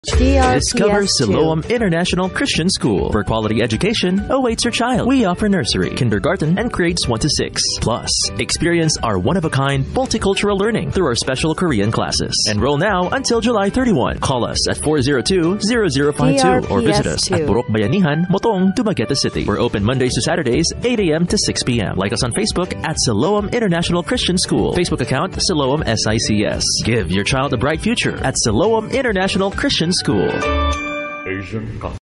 Siloam International Christian School Radio Ad Q3 2023 32s (Dumaguete)
Ito po ang Q3 2023 Provincial Radio Ad ng Siloam International Christian School sa Dumaguete City.

DRPS Two 4528: Siloam International Christian School Radio Ad Q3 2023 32s (Dumaguete)